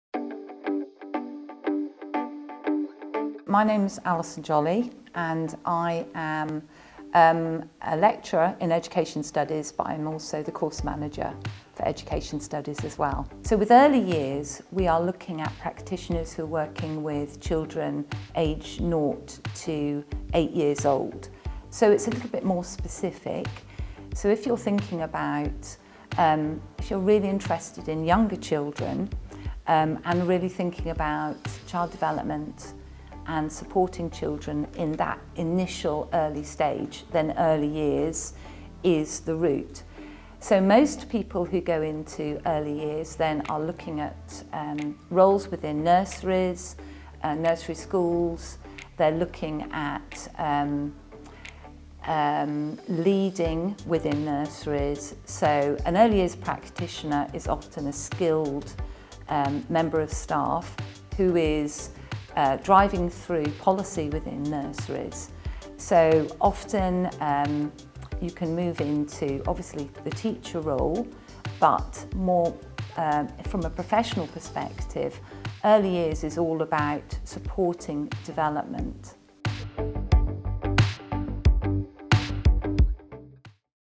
BA (Hons) Early Childhood Studies
Education lecturer Alyson discusses our BA (Hons) Early Childhood Studies degree.